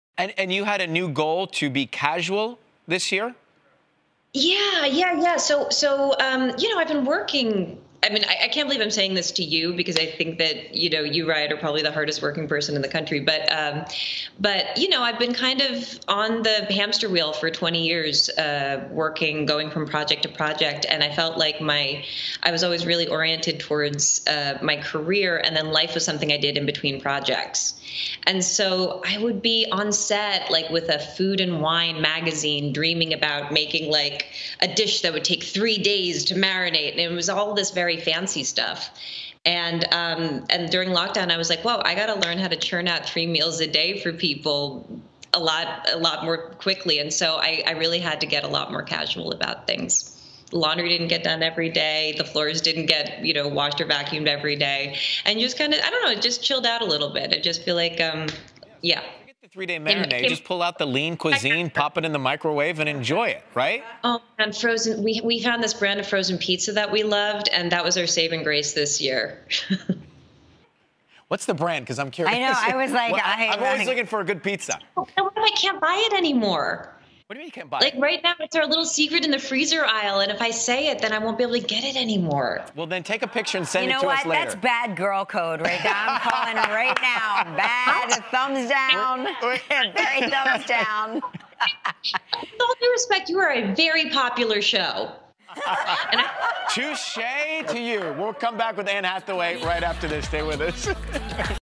Anne Hathaway's Resolution Is to Be More Casual
Anne Hathaway wants to chill out and be more casual this year.